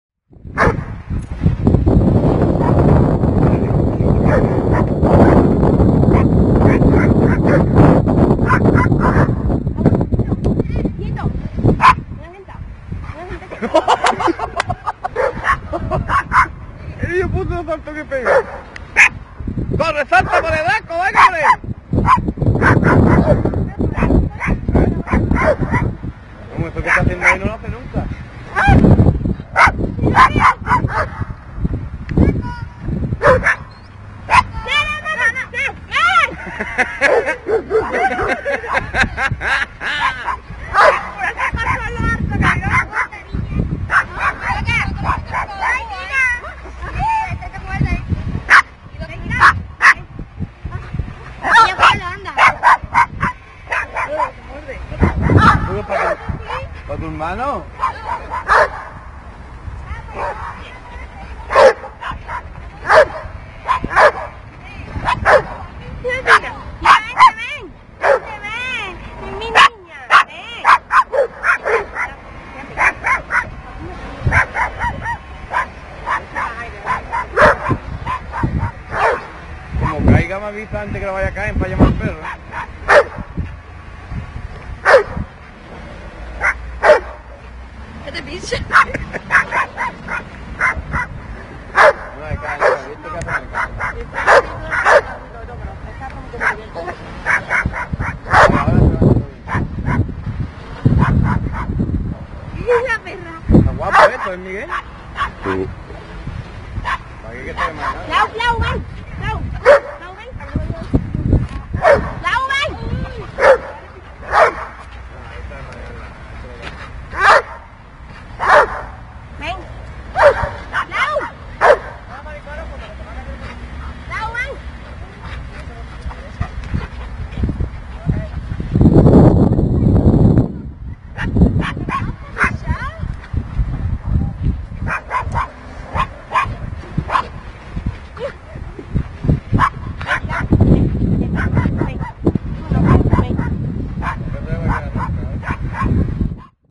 perro loco¡
este perrito se vuelve loco intentando cazr una cometa que vuela sobre el